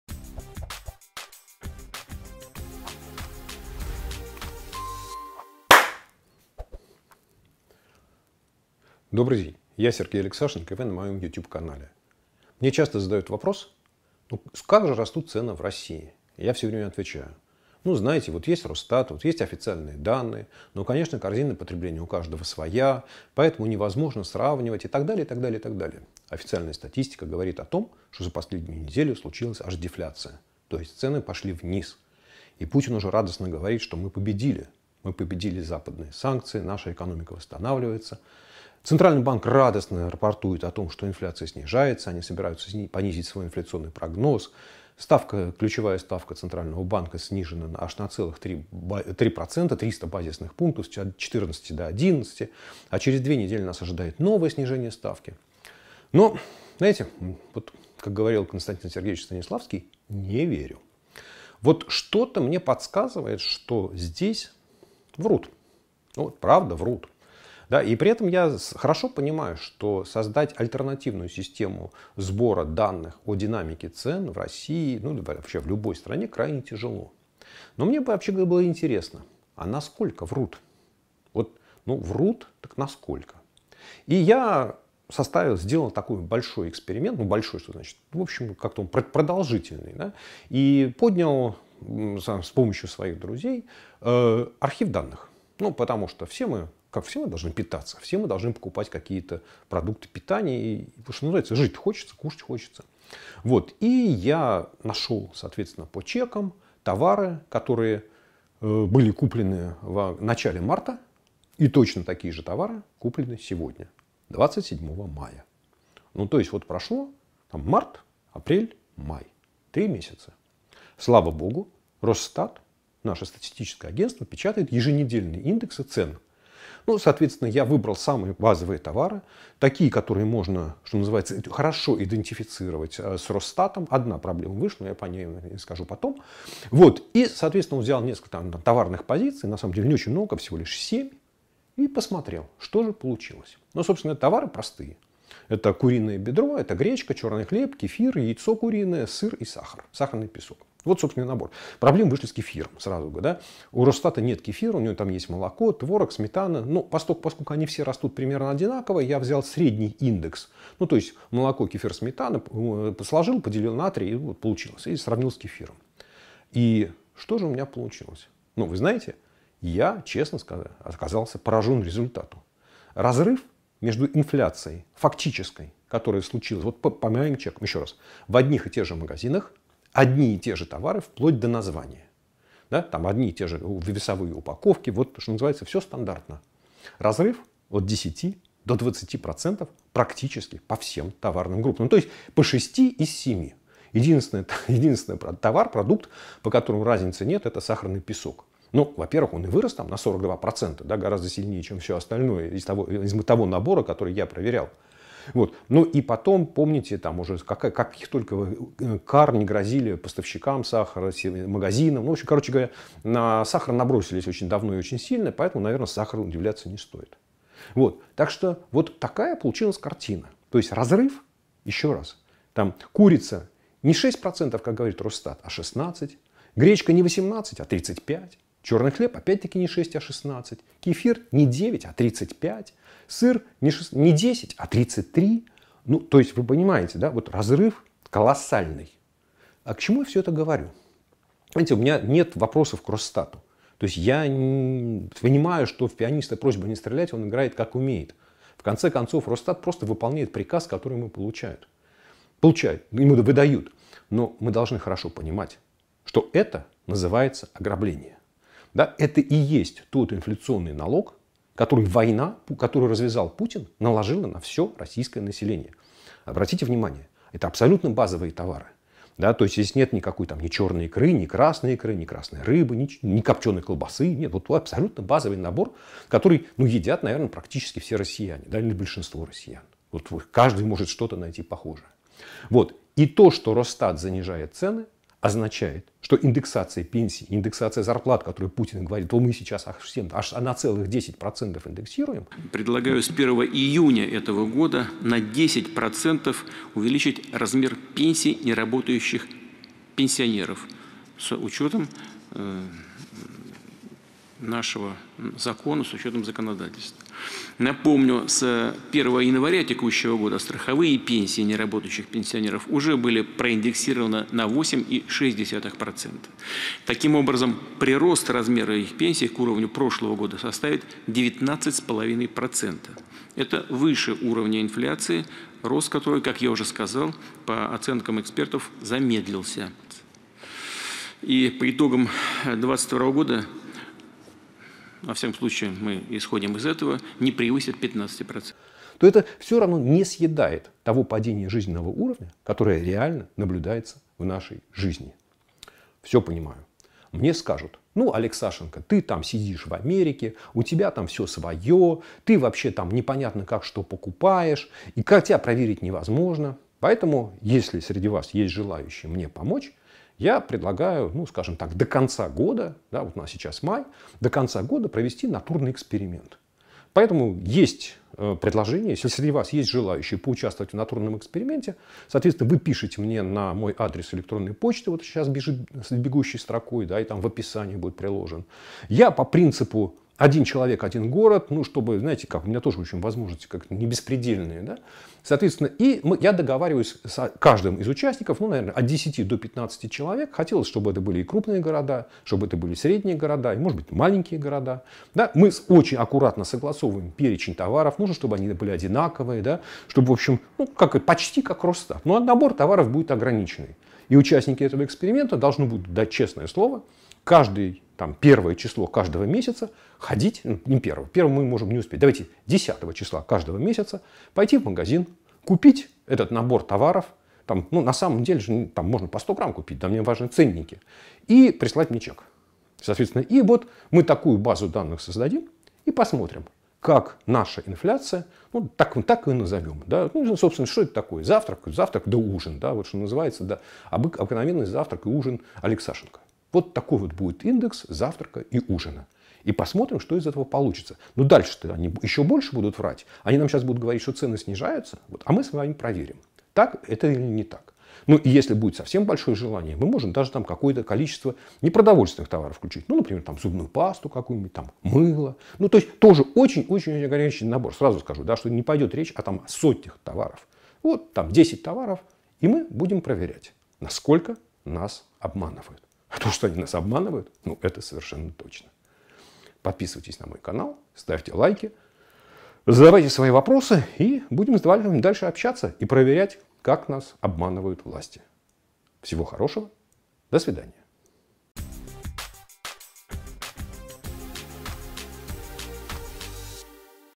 Сколько берёт Путин?
Инфляция это налог на войну.  Росстат неимоверно занижает рост цен. Я сделал небольшую проверку  и выяснил, сколько берёт Путин